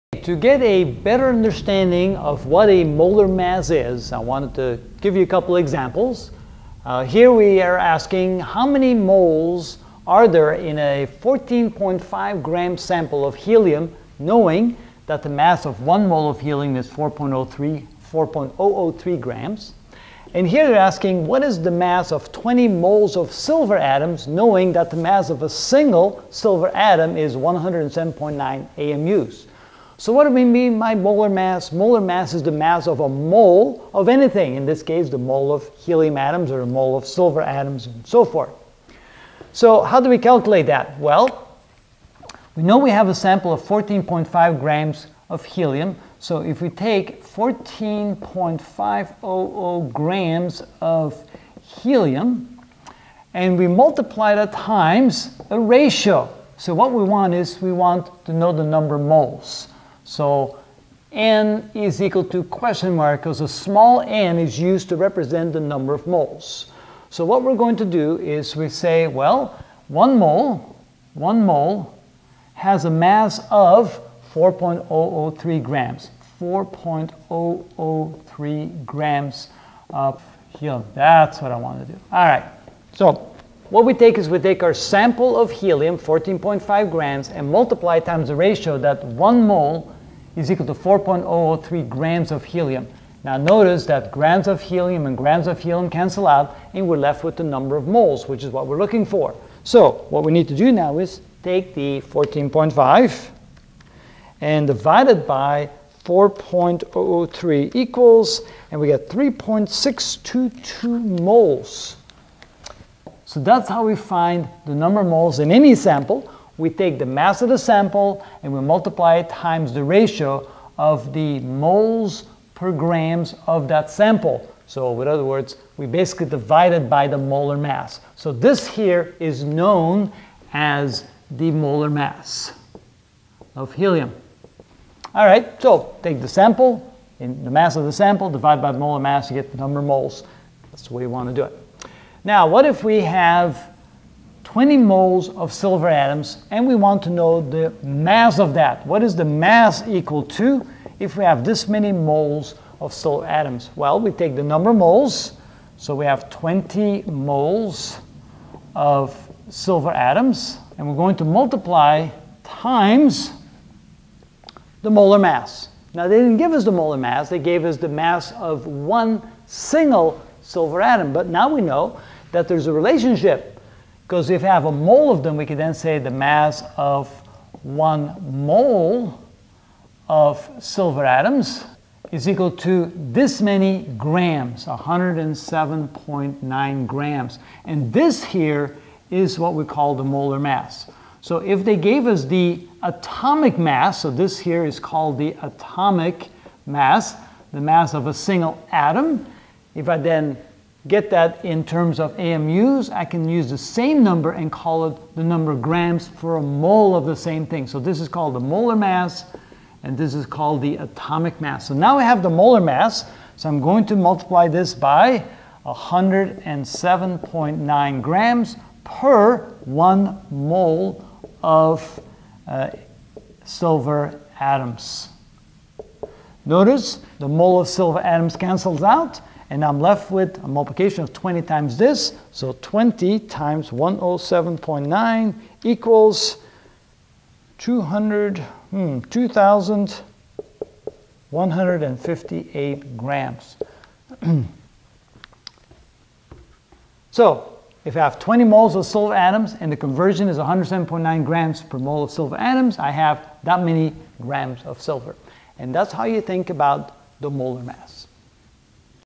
Chemistry - Atoms, Molecules, and Ions (5 of 23) Molar Mass
In this video I will explain and gives examples of what is a molar mass.